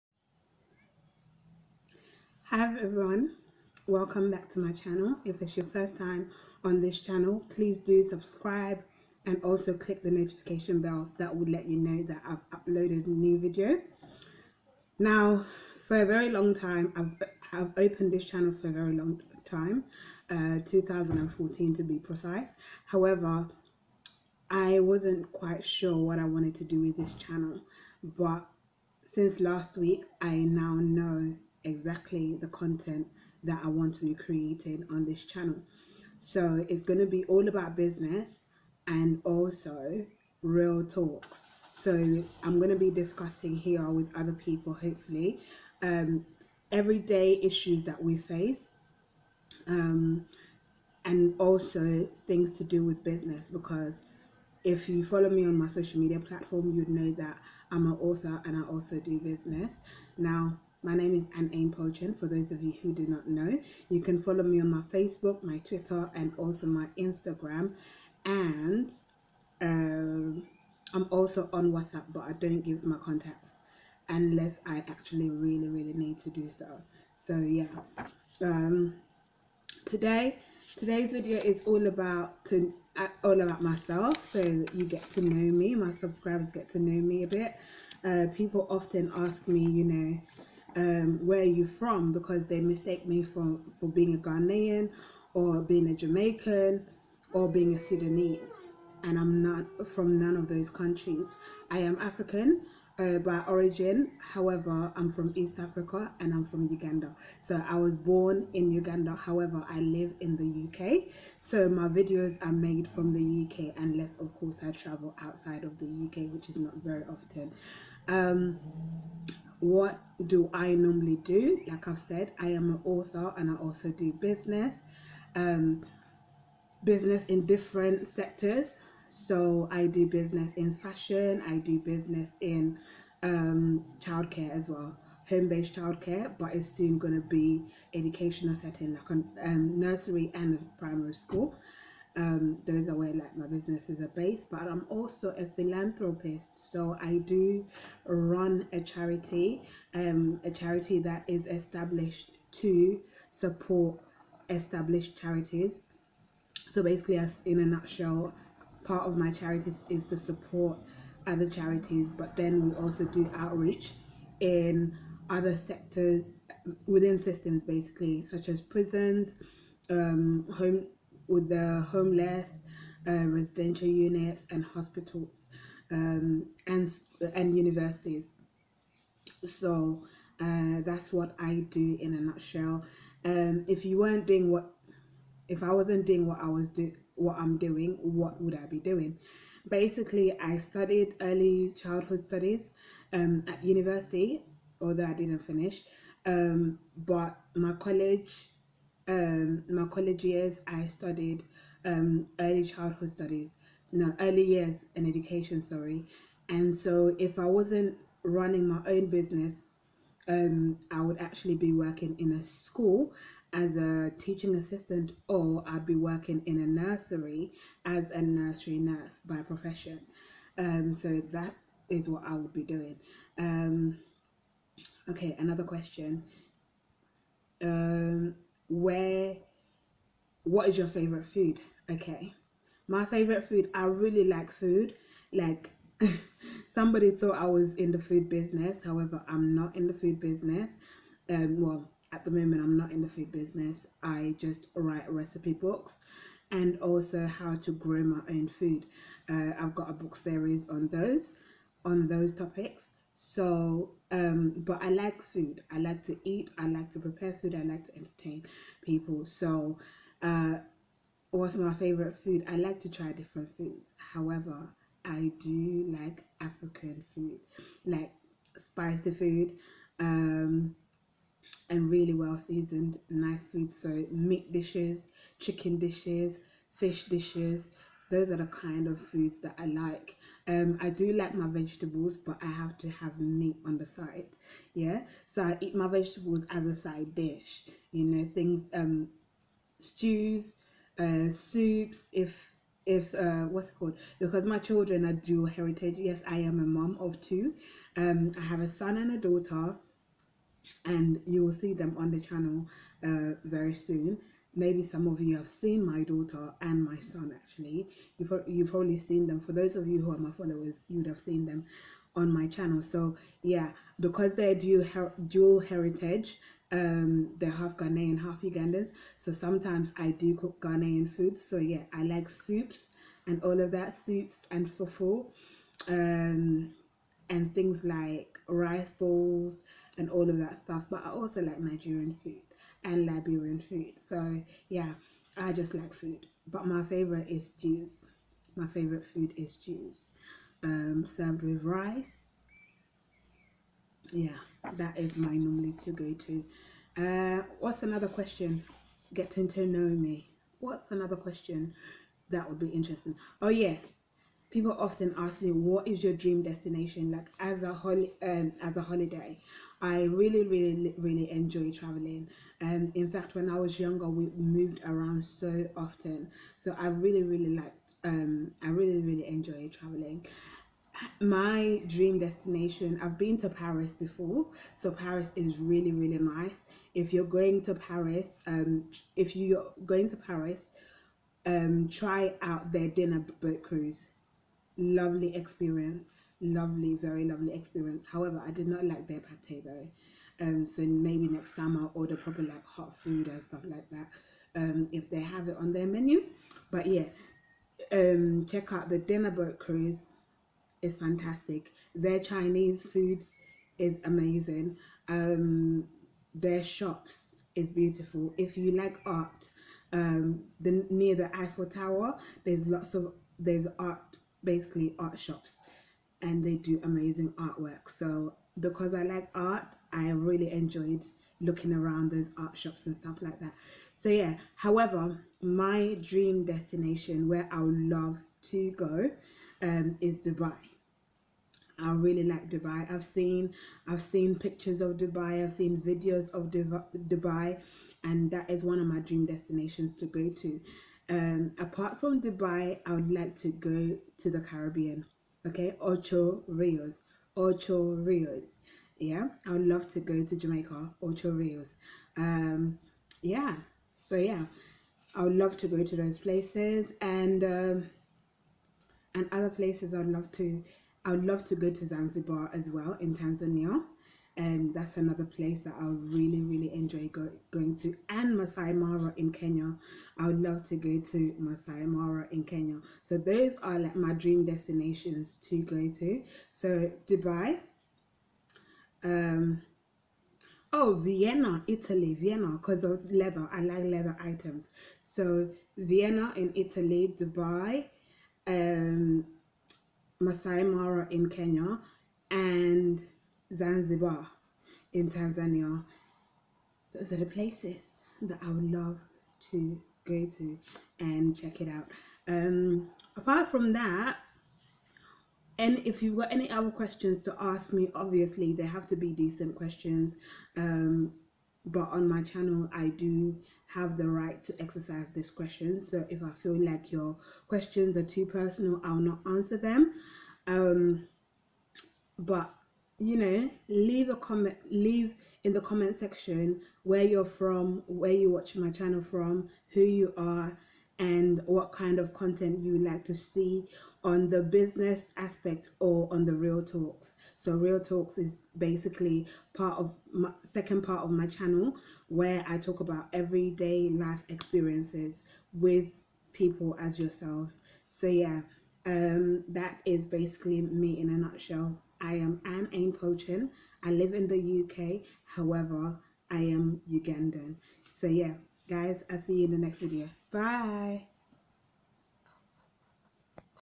GET TO KNOW OUR FOUNDER
People often ask me questions about myself or they make assumptions about me. So I thought I should share with you my audience a little bit about myself.